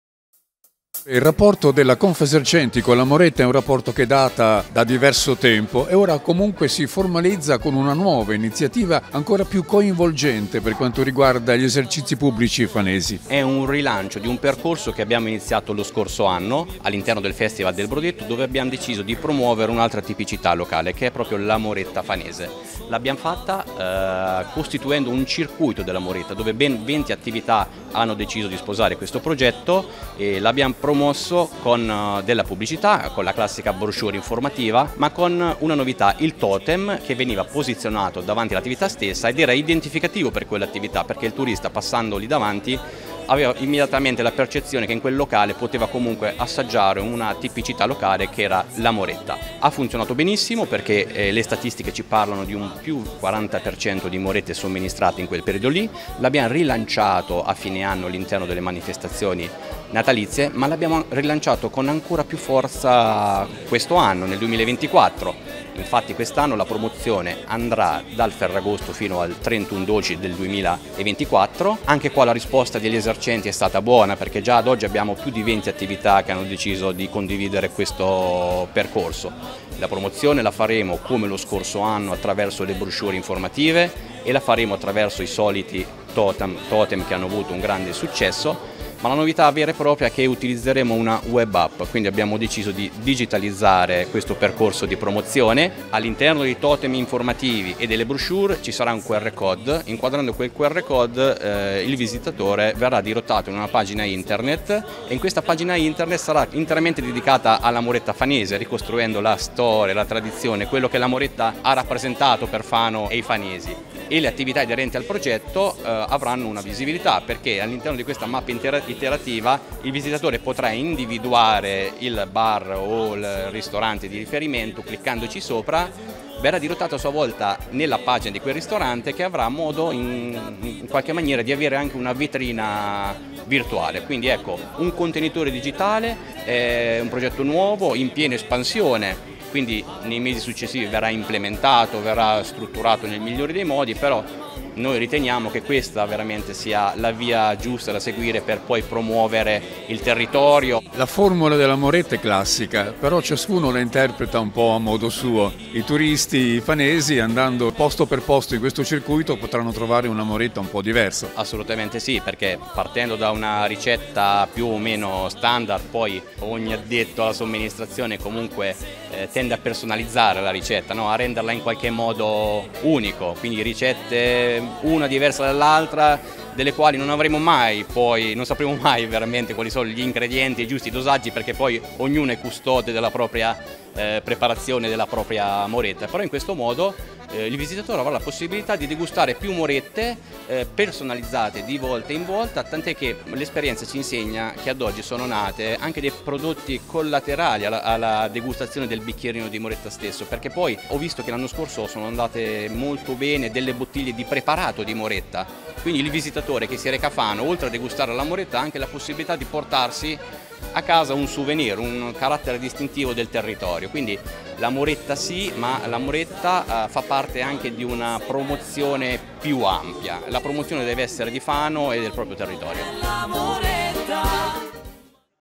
In 19 nel ‘circuito della moretta’ di Confesercenti
Parliamo della Moretta, che non è una semplice bevanda ristoratrice o un caffè corretto. E’ molto di più: è un simbolo del turismo fanese, un’espressione dell'enogastronomia tipica della nostra città. Ma dove si gusta una buona Moretta? A questo proposito ci ha pensato la Confesercenti organizzando “il circuito della Moretta” al quale hanno aderito in 19 tra caffè, bar e pasticcerie, contraddistinte da un totem che ne identifica la qualità.